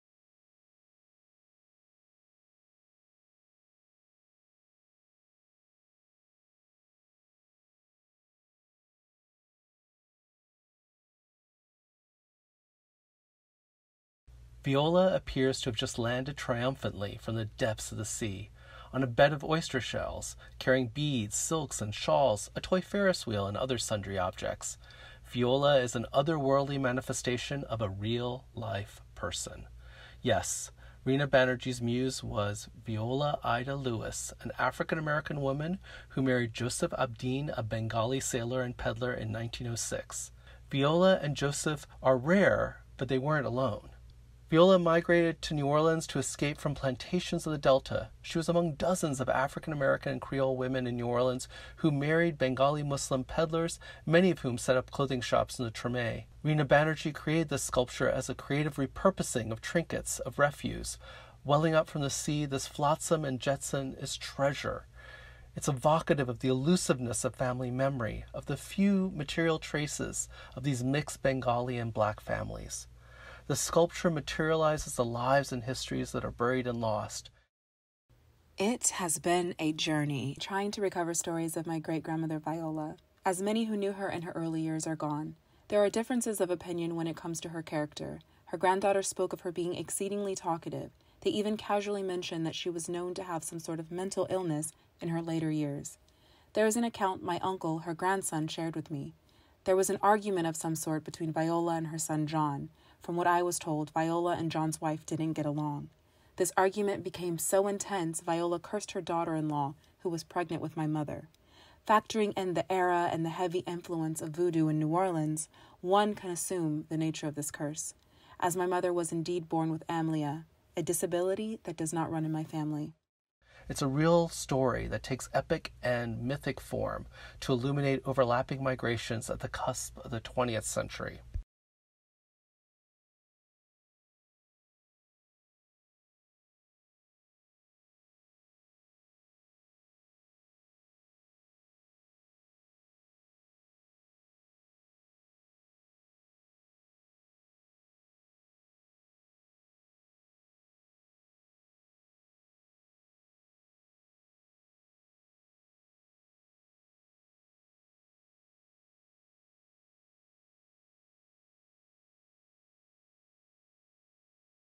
Culture Fix: Nayan Shah and Candace Browne on Rina Banerjee
We are bringing the exhibition and our programming to you! Our digital Culture Fix series offers brief explorations of exhibition objects.

Enjoy this Culture Fix with USC Professor Nayan Shah and Great Grandaughter of Viola Ida Lewis, Candace Browne.

Nayan Shah is Professor of American Studies and Ethnicity and History at USC. He is a historian with expertise in 19th and 20th century Asian American history and author of a book on South Asian migrants and their interracial relationships on the Pacific Coast and Southwest: Stranger Intimacy: Contesting Race, Sexuality and the Law in the North American West (University of California Press, 2011).